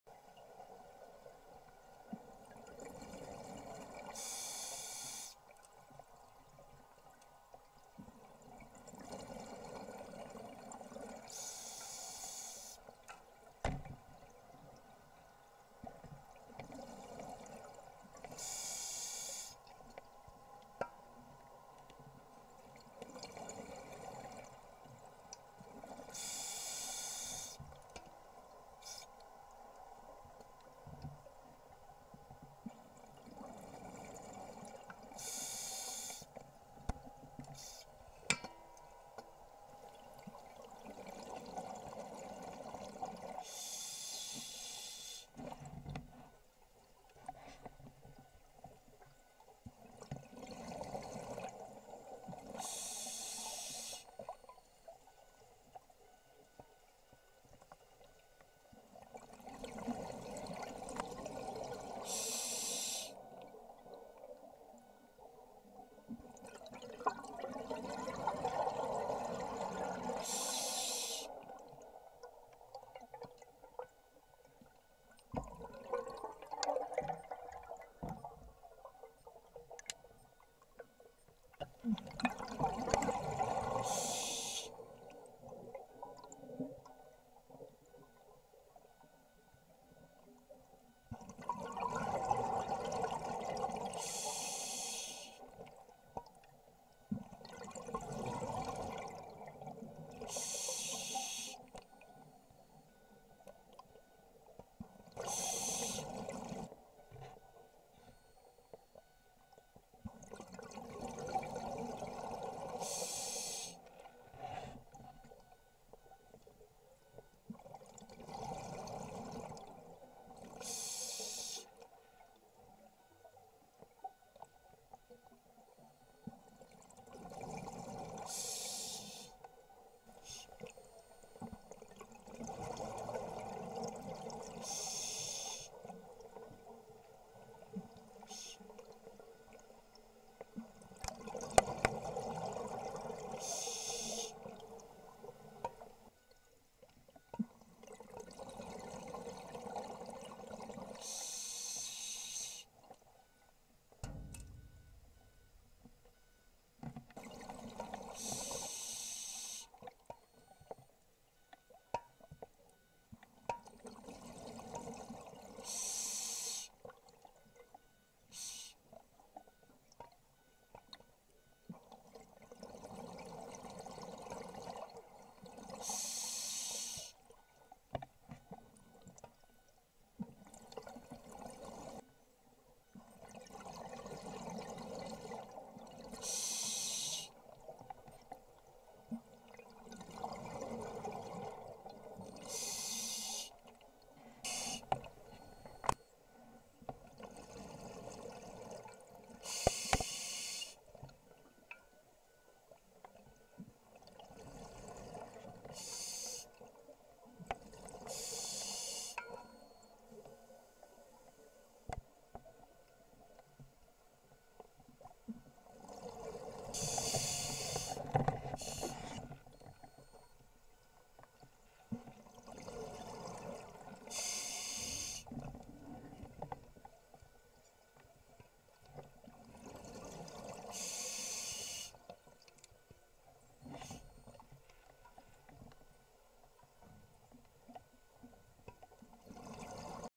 Annapolis Dive
Drivers explore HMCS Annapolis, now an artificial reef at the bottom of Halkett Bay in Howe Sound.
Video by Chris Straub, Artificial Reef Society of B.C.